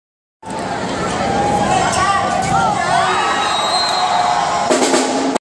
Pldt blocks Systema
These are clips taken from my mobile phone. Short spurts of fantastic and not so fantastic men's volleyball. Enjoy!